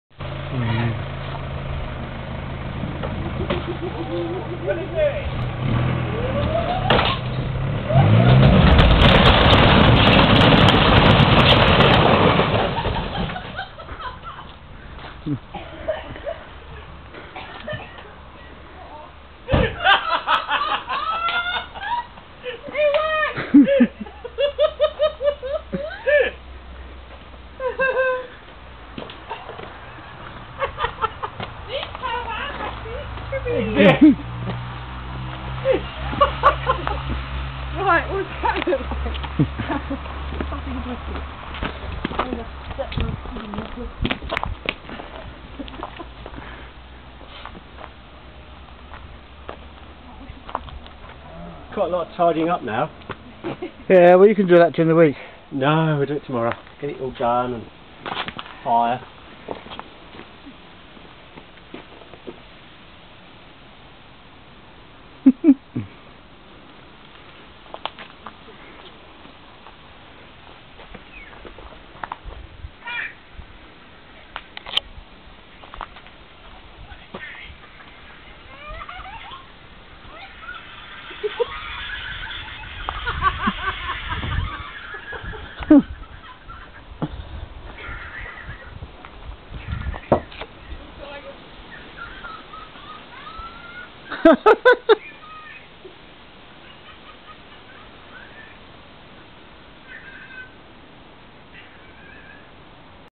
Caravan
Fun at Fairlight with a caravan.